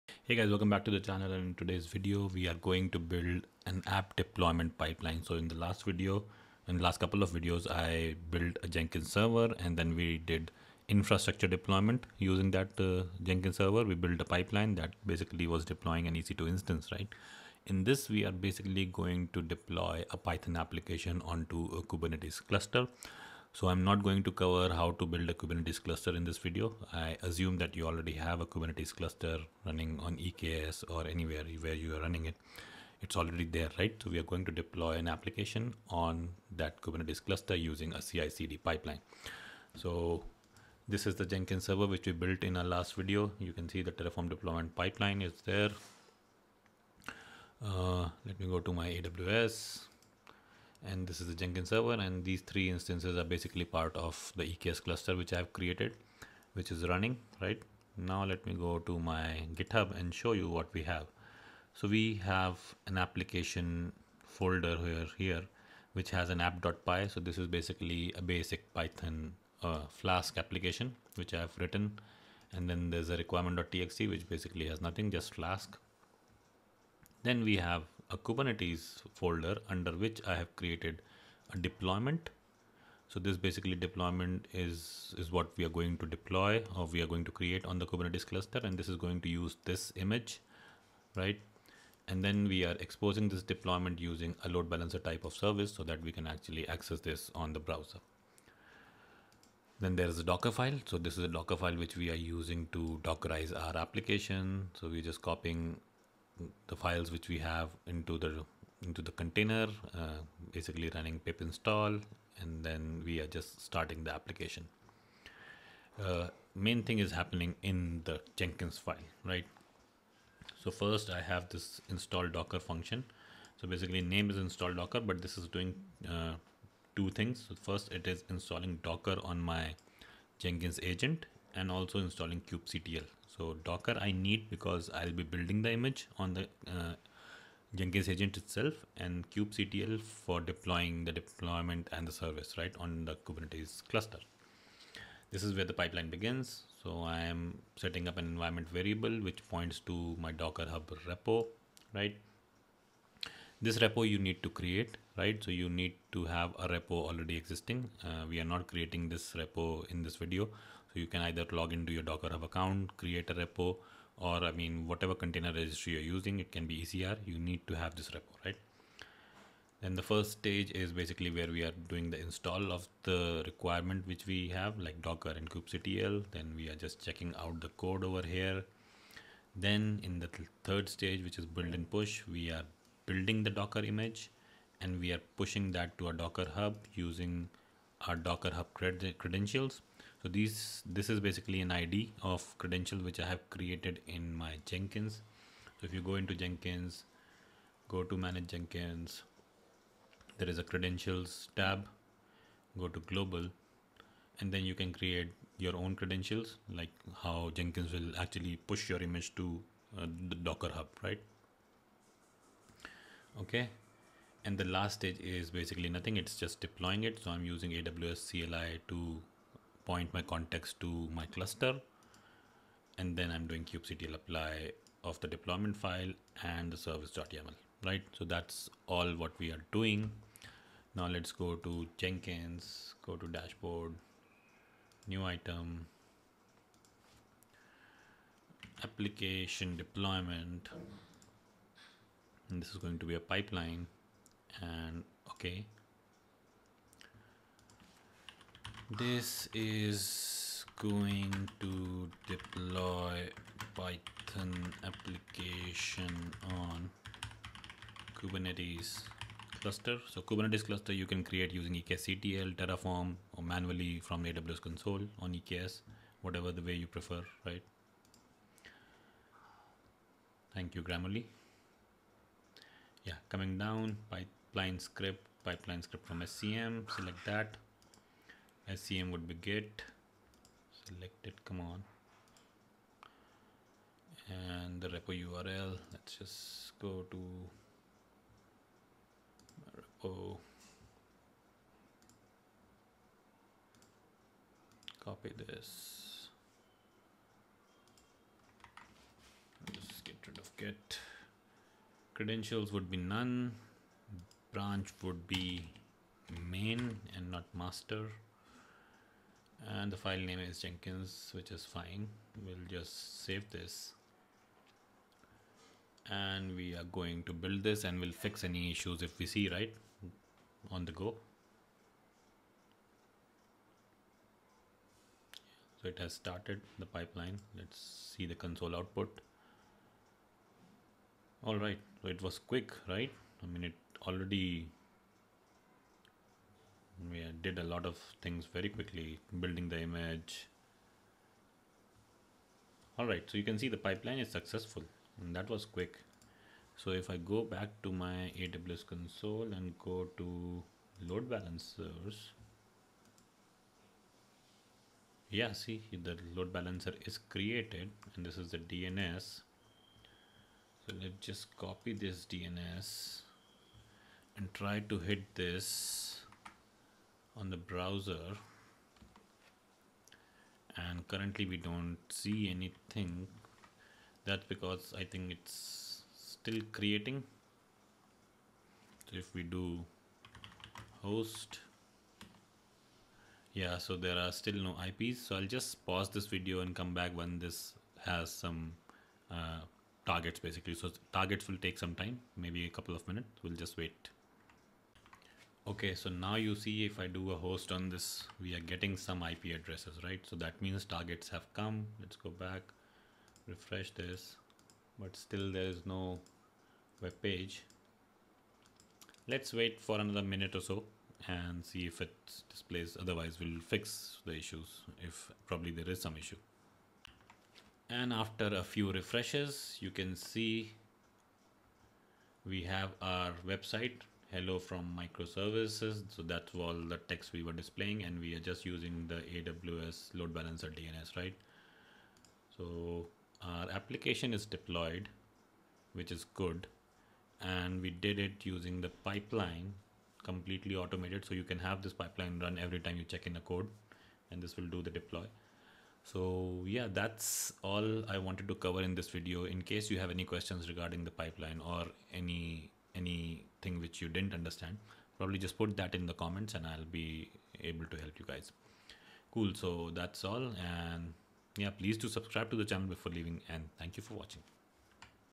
How to build an app deployment pipeline? Deploying a python application onto a Kubernetes cluster.
🚀 Learn How to Build an App Deployment Pipeline! 🚀

Welcome to our YouTube tutorial where we'll guide you through the process of building an efficient app deployment pipeline and deploying a Python application onto a Kubernetes cluster! 🐍⚙️

In this tutorial, we'll cover:

Setting up a robust deployment pipeline from scratch
Building and packaging a Python application for deployment 📦
Configuring and deploying the application onto a Kubernetes cluster ☁️
Automating the deployment process for seamless updates 🤖

Whether you're a beginner or an experienced developer, this tutorial is packed with practical insights and hands-on demonstrations to help you master the art of app deployment. 🛠️

Join us in this exciting journey as we simplify complex concepts and empower you to deploy applications with confidence. 🔔✨